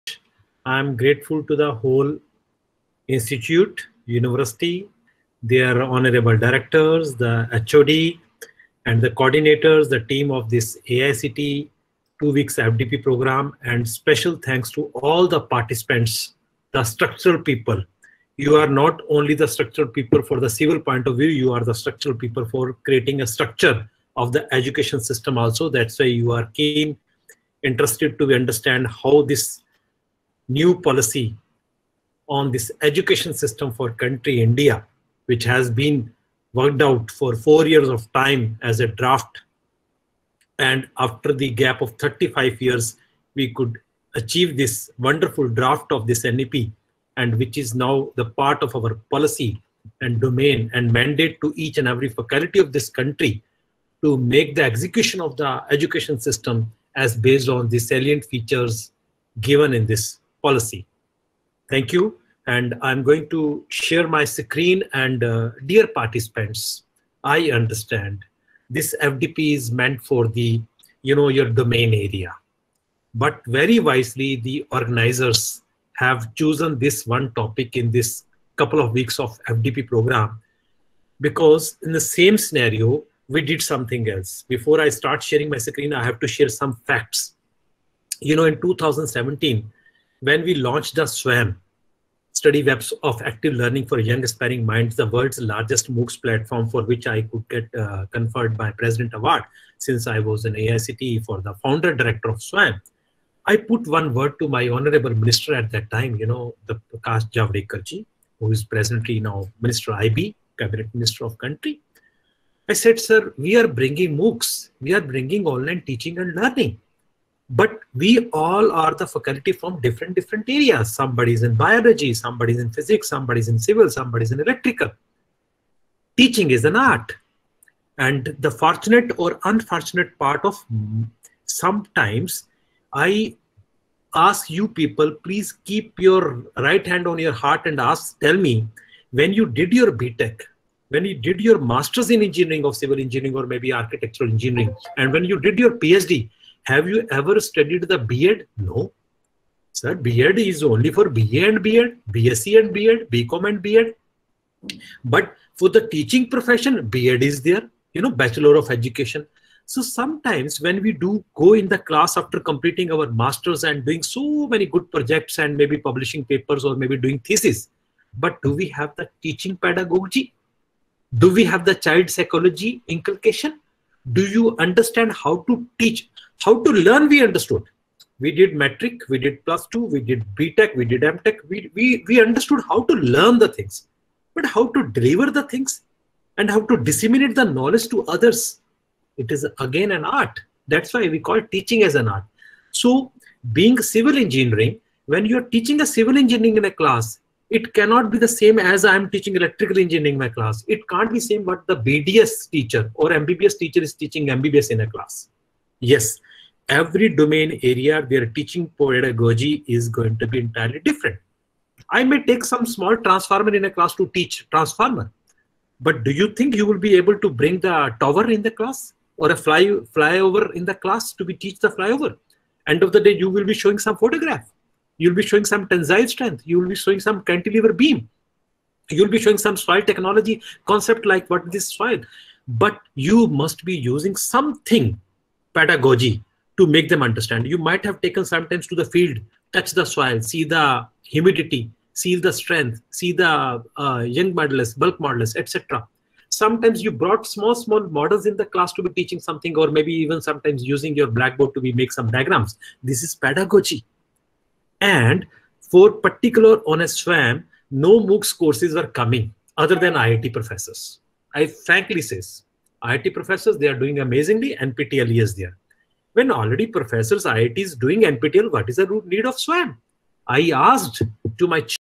AICTE - SKCET - Civil Engineering | Online FDP | Day 6 - 15.May.2021 | Session 3
SKCET_Dept of Civil Engineering
AICTE - Sponsored Two week FDP  on “Structural Assessment, Repair and Rehabilitation of Structures - SERIES 1”@ Department of Civil SKCET

Day -6: Session #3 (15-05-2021 AN )

Resource person:
Dr. Manpreet Singh Manna
Former Director, All India Council for Technical Education
(AICTE), Govt. of India.

Topic: National Education Policy 2020

Session Highlights
🔈 Holistic Multidisciplinary Education with multiple entry/exit options
🔈 Setting up of Multidisciplinary Education and Research Universities(MERUs)
🔈 ‘Light but Tight’ regulation
🔈 Revamping curriculum, pedagogy, assessment and student support.
🔈 Setting up of National Research Foundation (NRF)
🔈 Internationalization of Education
🔈 Creation of an autonomous body, the National Educational Technology Forum (NETF), NAC, NHERC